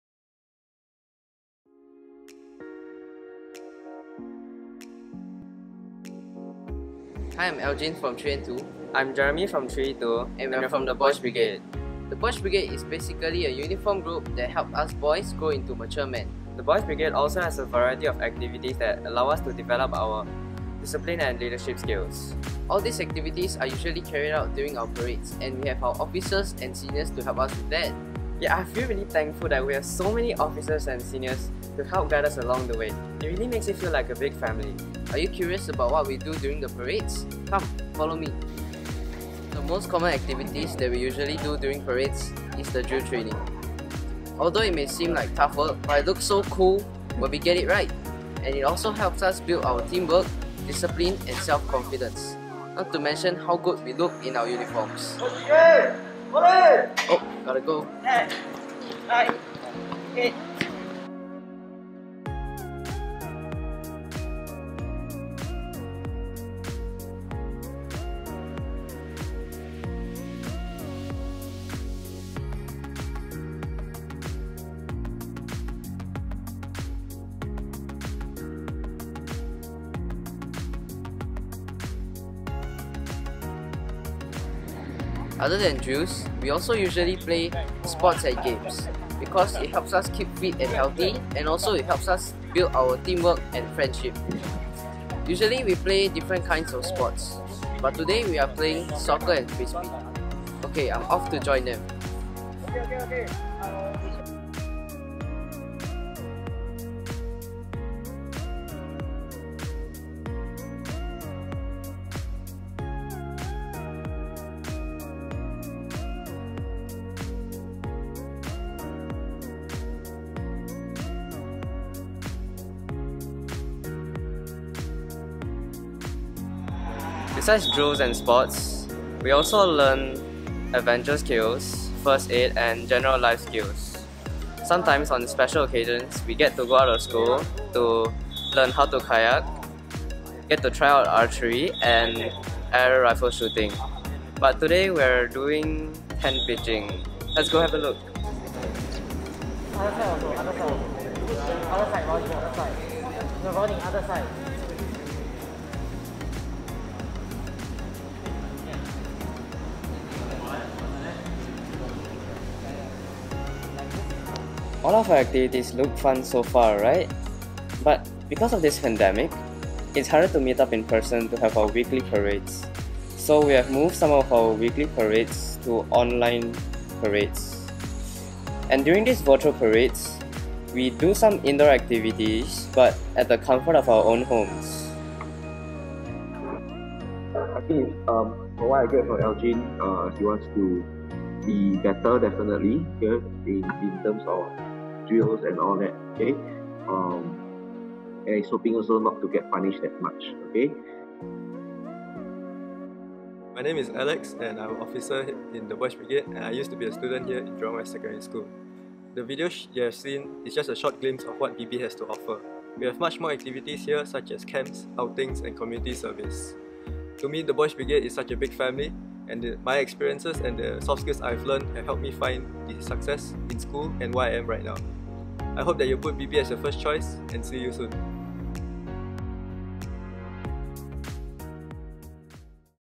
Introducing The Boys' Brigade 85th Singapore Company (Jurong West Secondary School)
This video was created for Jurong West Secondary School's CCA e-Open House to introduce The Boys' Brigade's 85th Singapore Company.